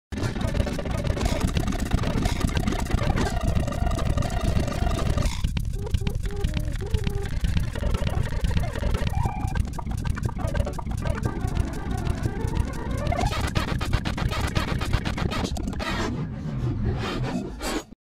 Morrissey extreme speed painting by stevie jacko
this painting black and white acrylic on canvas took about a week in all about 4 hours a day , i am a very slow artist but i like the results i get and love taking my time especially when i enjoy the subject